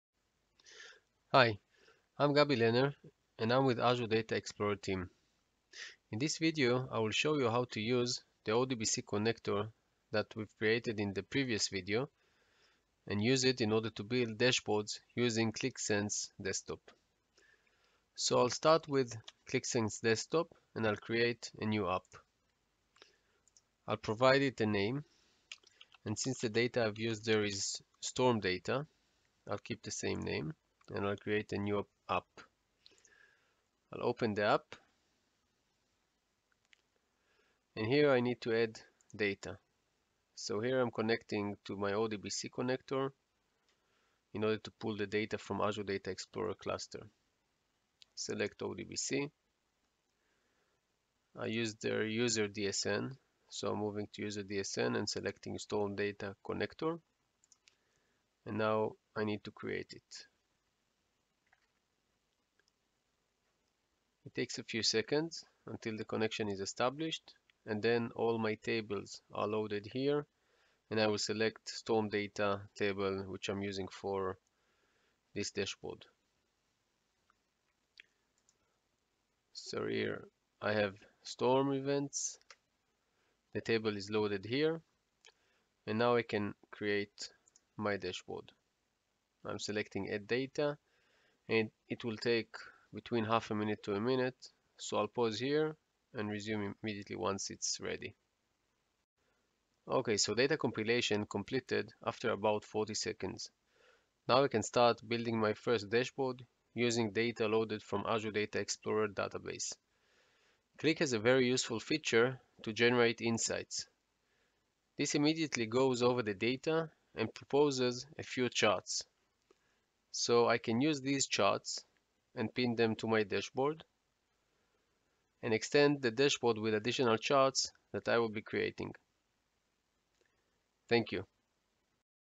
Qlik dashboards with Azure Data Explorer
Interested in building beautiful Qlik dashboards on top of big data hosted in Azure Data Explorer? In this video I will demo how to create Qlik Sense dashboards powered by Azure Data Explorer.